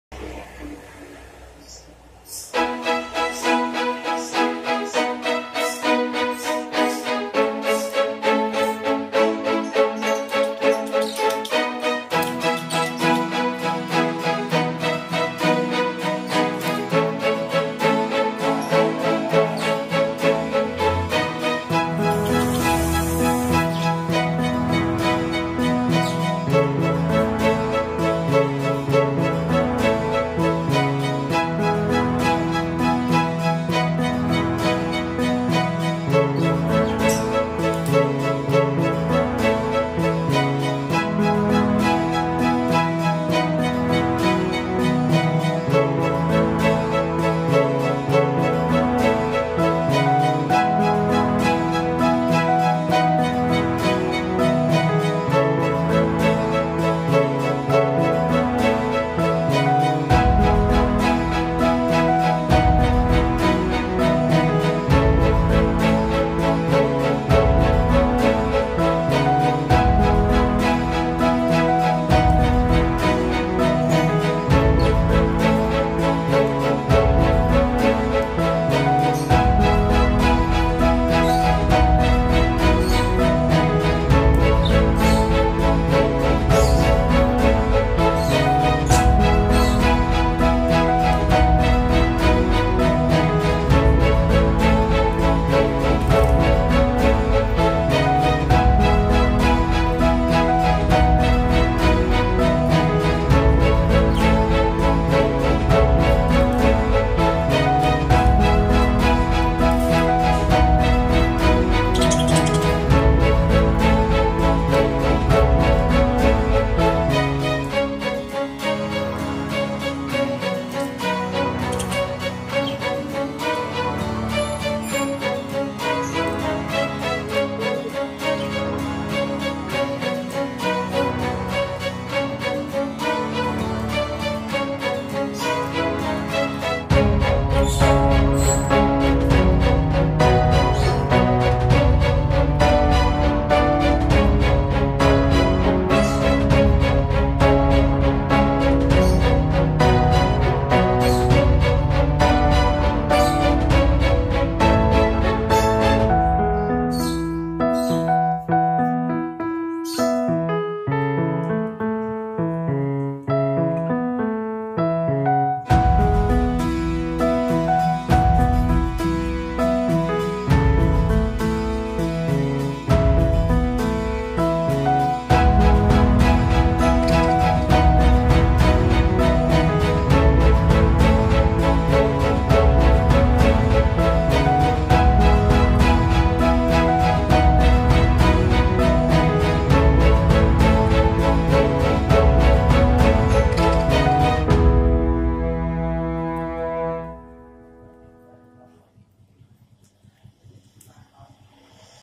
Burung Parkit Jinak Di Tangan
burung parkit bisa di jinakkan dan bisa di ajak untuk bermain